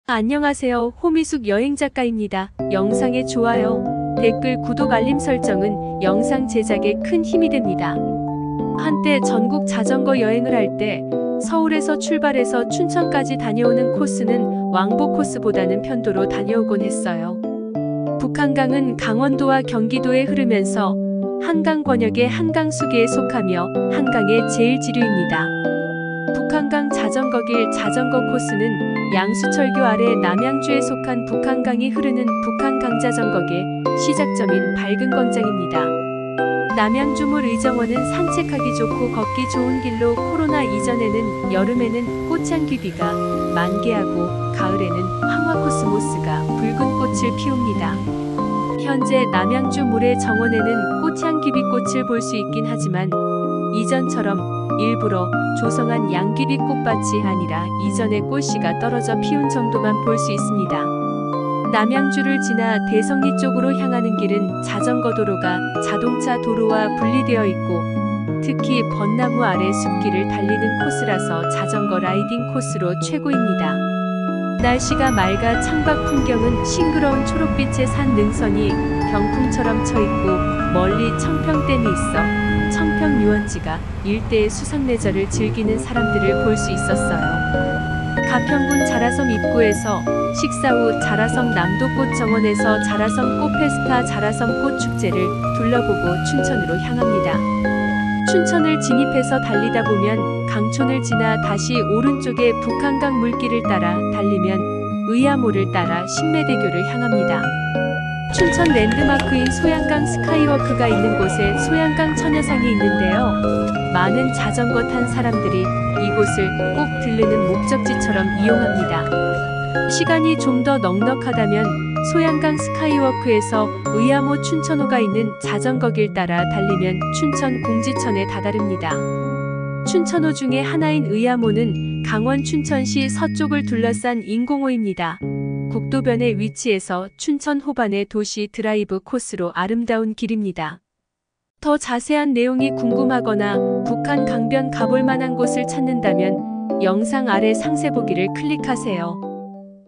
북한강 자전거길 자전거여행코스 남양주에서 춘천까지 대성리국민관광지
서울 고덕동 출발 경기도 남양주-대성리-청평유원지-가평 남이섬-춘천의암호-춘천스카이워크-공지천유원지-춘천 레고랜드코리아 리조트-춘천 막국수 큰집닭갈비막국수- 서울 귀가